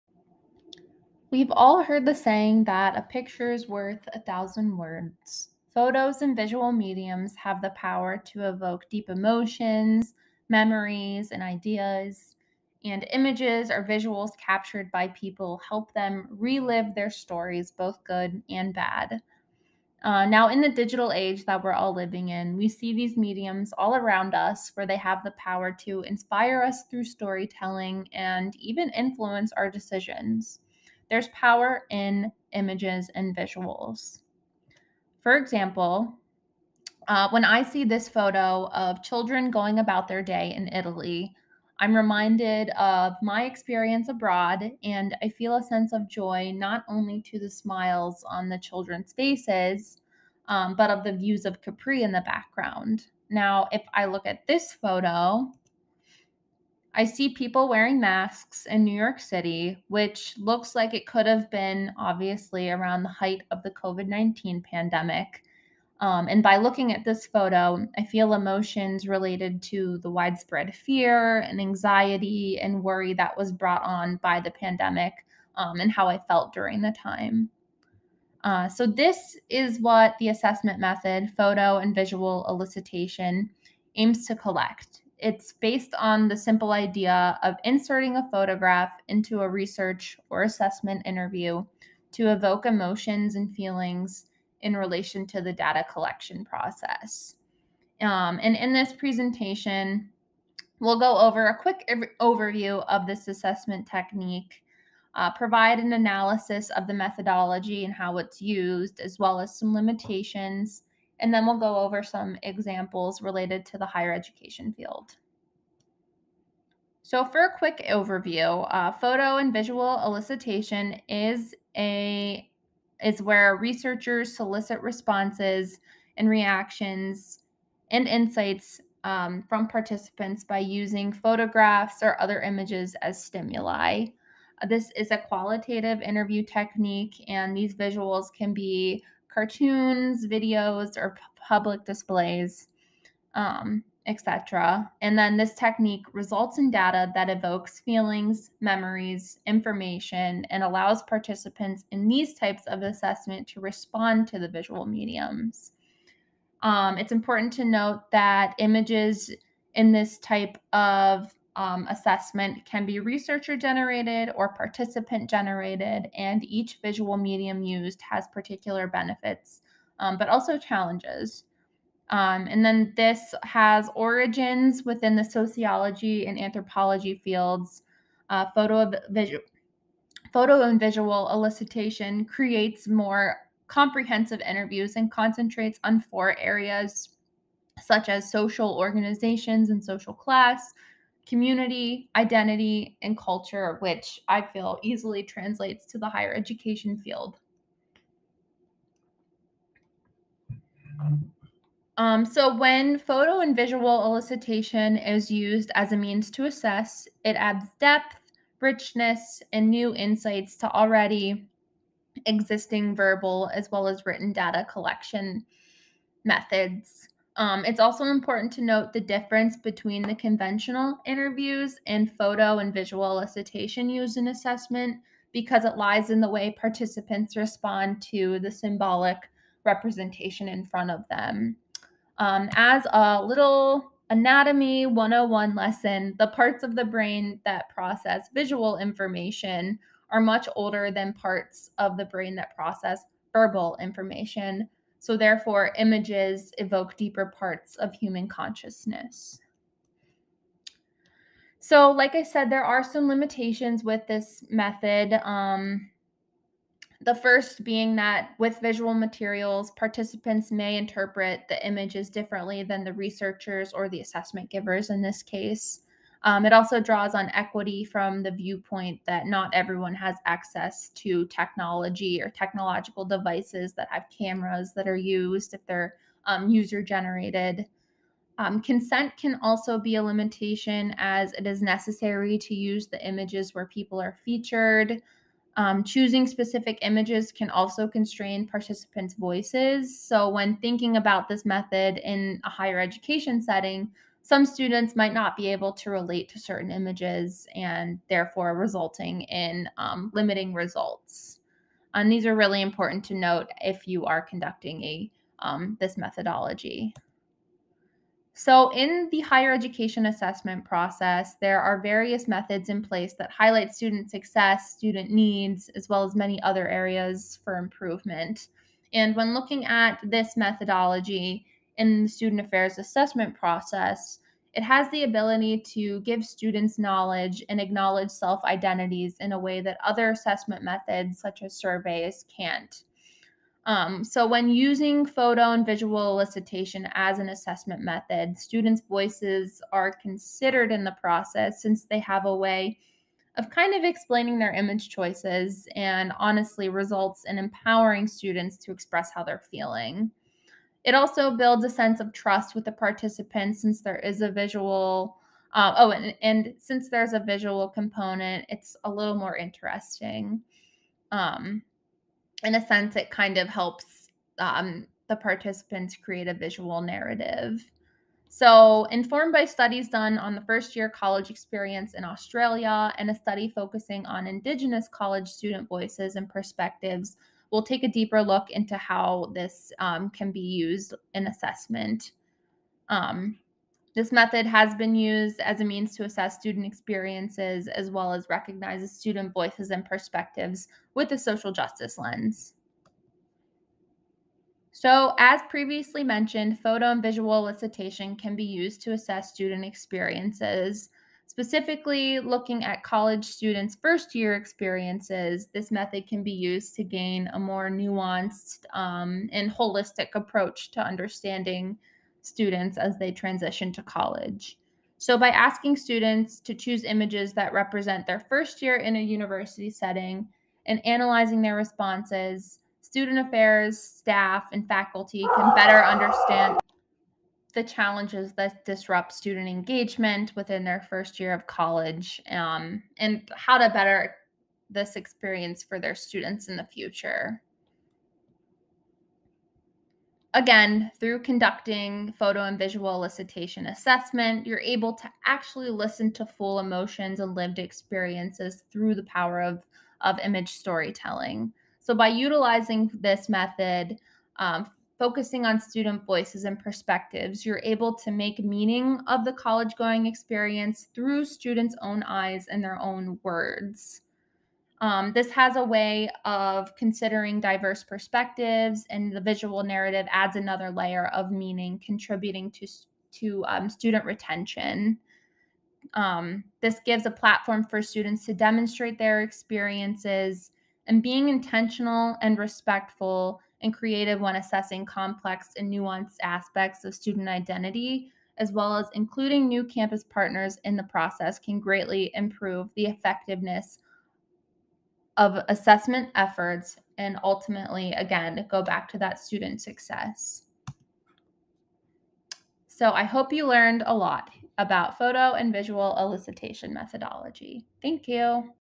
HEd Talk: Photo and Visual Elicitation Methodology
In the presentation, I go over the research/assessment method, Photo and Visual Elicitation.